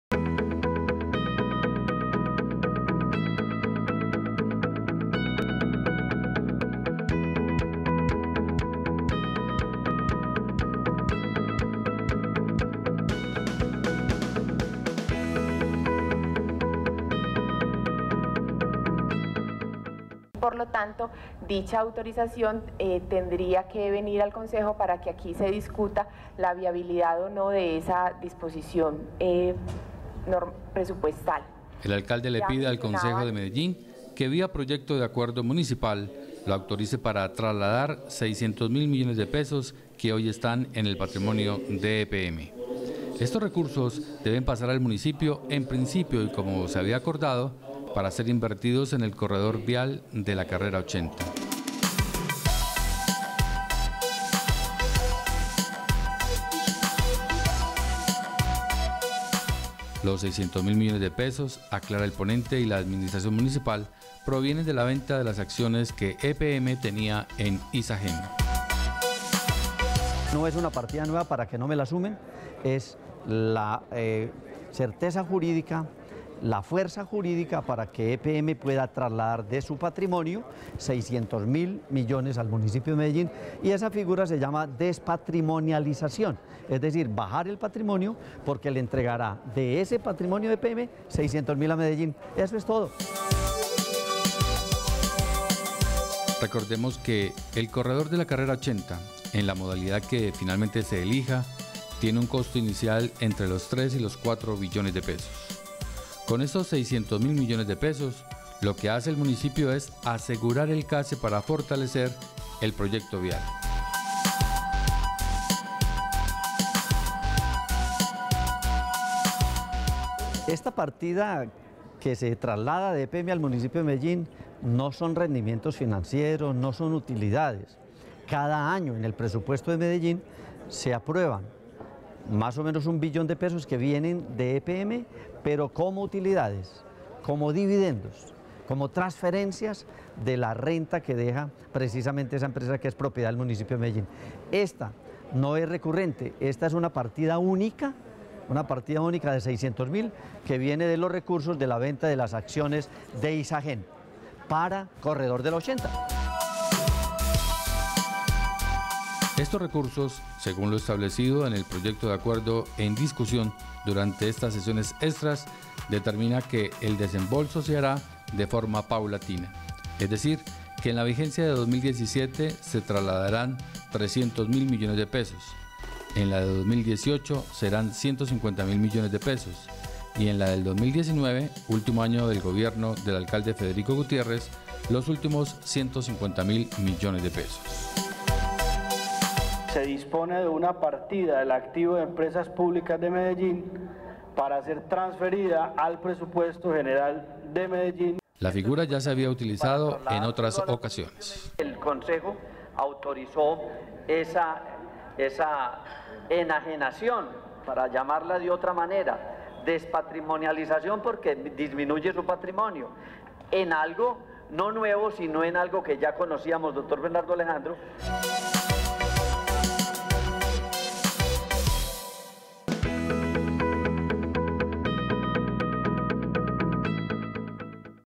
De Acuerdo: Partidas de EPM al Municipio se discuten en el Concejo
El Alcalde Federico Gutiérrez Zuluaga solicitó al Concejo de Medellín que analice vía Acuerdo el traslado de 600 mil millones de pesos del patrimonio de Empresas Públicas de Medellín, EPM. Estos recursos provienen de la venta de acciones en Isagen y serían usados en el corredor vial de la 80.